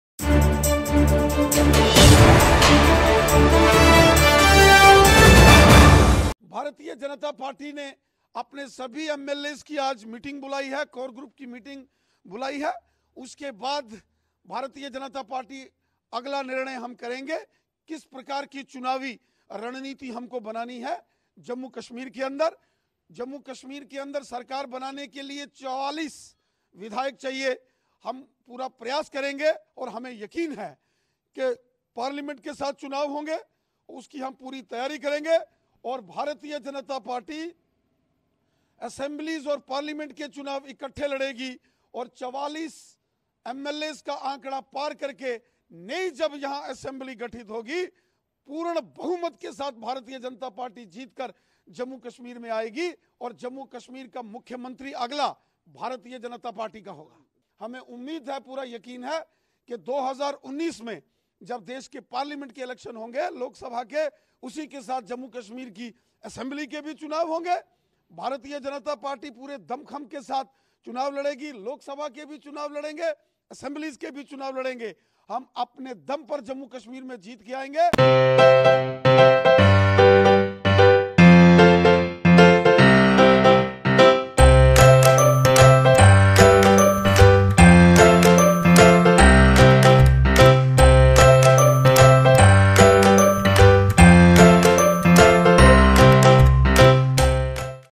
Assembly elections in J&K should be conducted with Parliament elections: Ravinder Raina
Jammu, Nov 22 : While talking about the Assembly elections in Jammu and Kashmir, Bharatiya Janata Party (BJP) state president Ravinder Raina said, “The BJP has called for an important meeting with all its MLAs today and we will decide the future course of action.